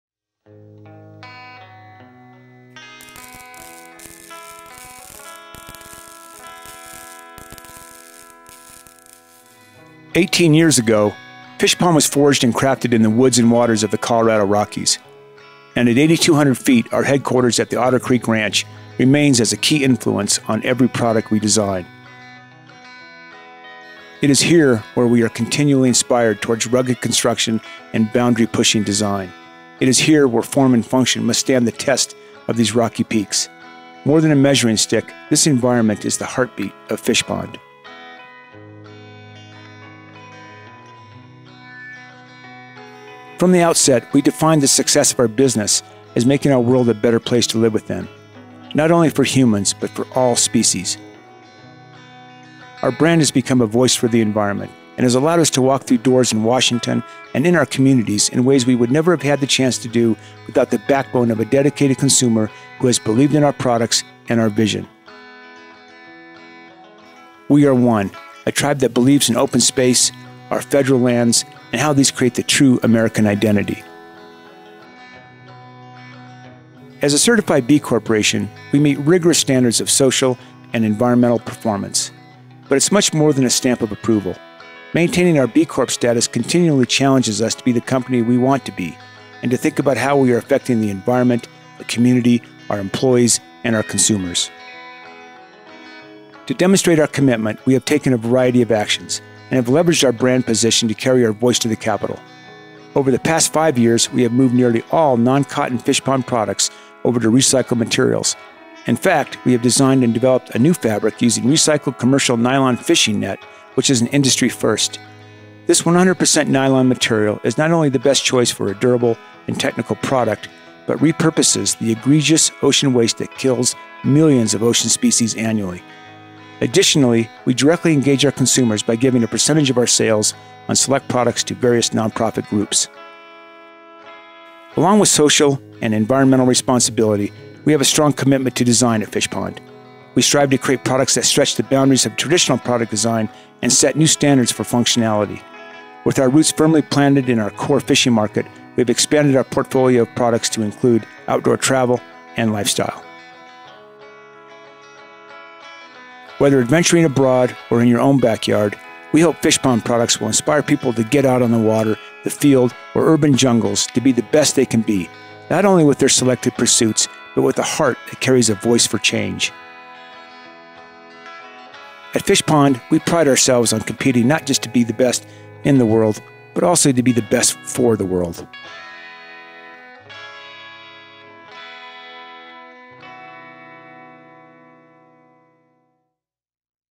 The Heartbeat of Fishpond Fly Fishing Packs and Bags
At Fishpond, we have a passion for creating products that stretch the boundaries of traditional product design and set new standards for functionality. With our headquarters in the high mountains of Colorado, we are inspired by the land and water that allow us to focus on rugged construction and enable us to evaluate the performance we build into each and every one of our designs. This is our story and the heartbeat of Fishpond.